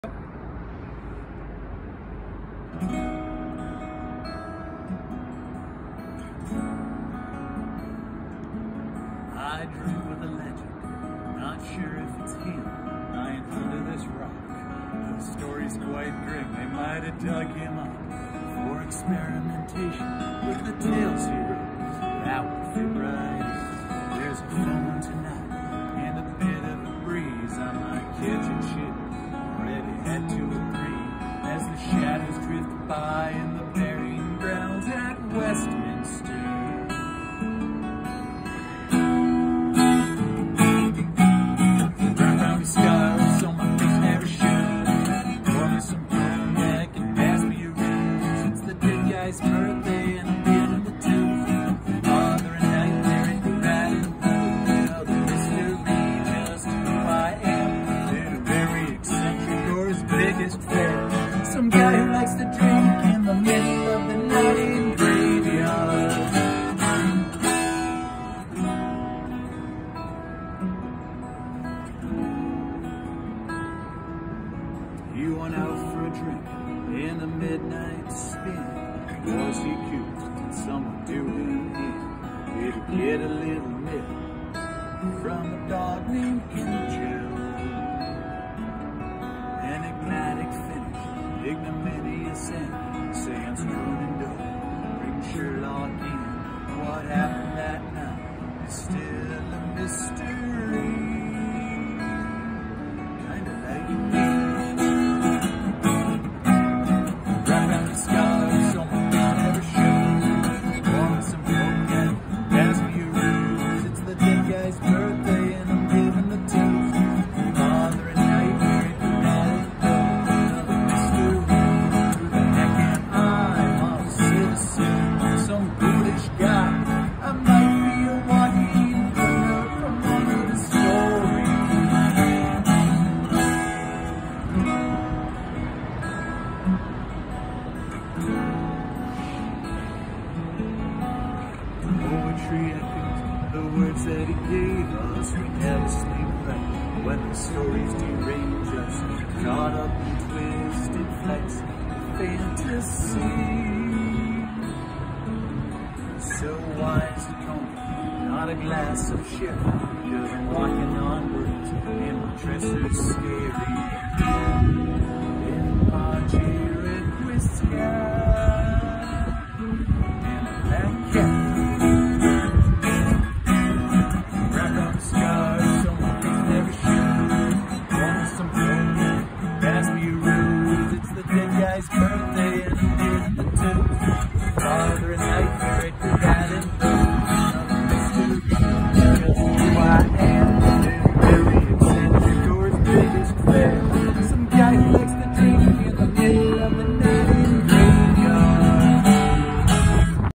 The Poe Toaster - at Edgar Alan Poe’s Grave!
Live performance of The Poe Toaster while walking from his cenotaph to his grave.  Filmed on location in the Westminster graveyard in Baltimore.  Happy birthday Edgar Alan Poe!